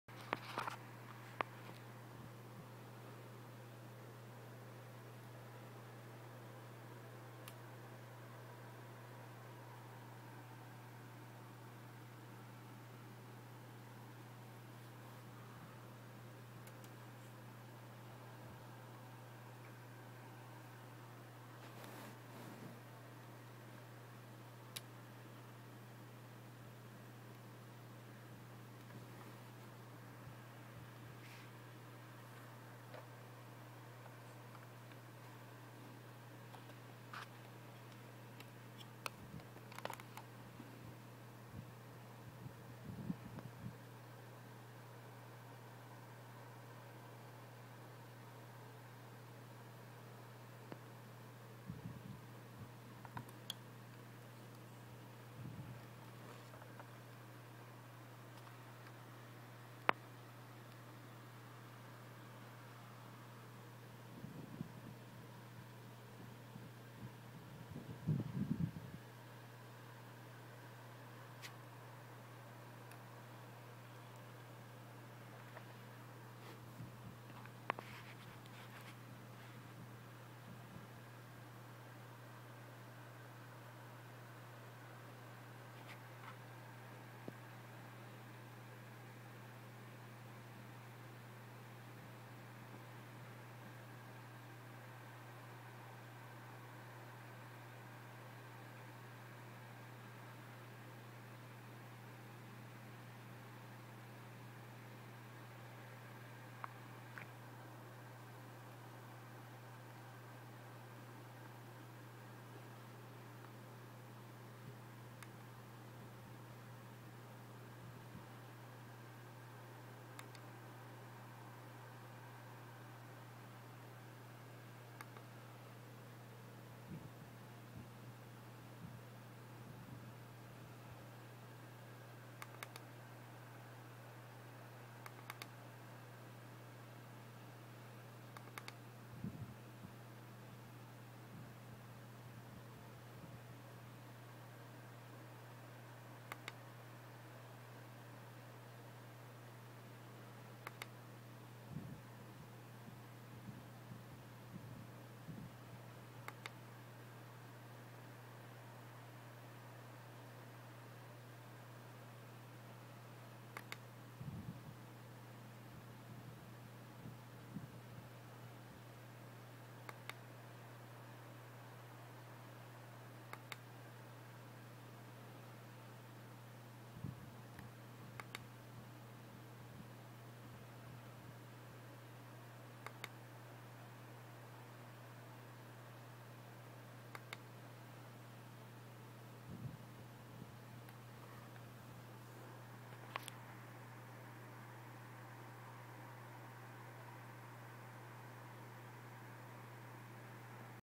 1/18 CHP K-9 Unit
California Highway Patrol K-9 unit white slick top...included is the Vector Lightbar with rotating LEDs!!! (and traffic adviser on back) total of...78 LEDs!!! Will be installing 4 mode siren later and fiber optics into dash and center console.